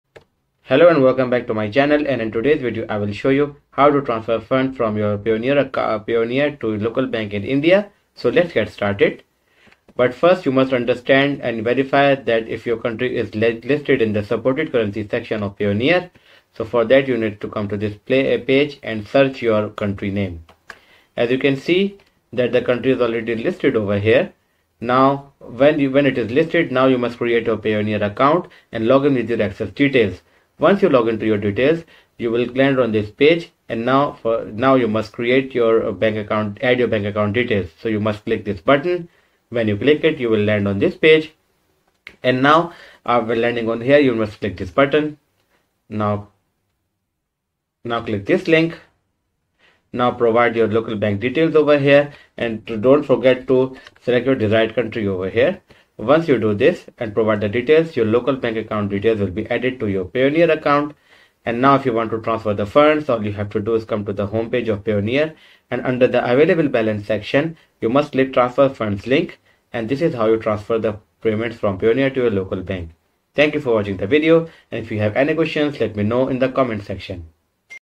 ✅ How To Transfer Money From Payonner Account to India Bank Account (Full Guide)
How To Transfer Money From Payoneer Account to India Bank Account (Full Guide)

Embark on a seamless international money transfer journey! This detailed guide takes you through the process of transferring money from your Payoneer account to an India bank account. Whether you're a freelancer, entrepreneur, or involved in cross-border transactions, this step-by-step tutorial ensures a smooth and informed financial transaction.

Key Highlights:
🌐 In-depth walkthrough on transferring funds from Payoneer to an India bank account
💡 Tips and strategies to optimize your international money transfer experience
📊 Understand the fees and exchange rates for well-informed decision-making

Join us in demystifying the process of transferring money to an India bank account using Payoneer.